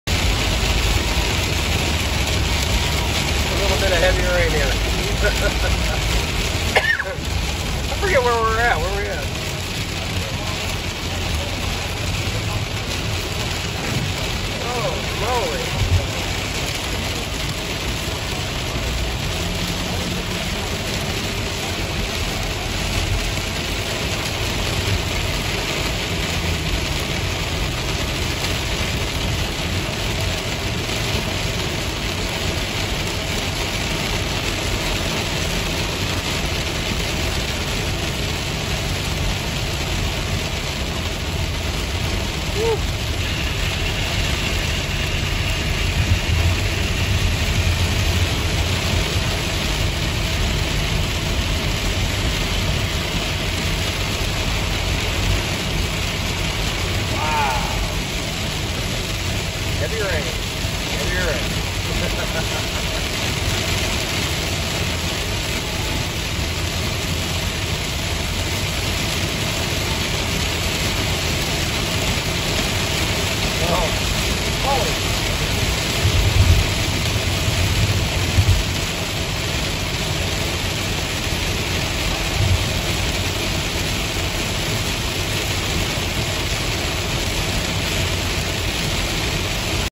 Heavy October Rain - October 12, 2021
Hitting heavy rain on Tuesday, October 12, 2021 at about 3:45 p.m. on northbound Interstate 135 just south of Hesston, Kansas.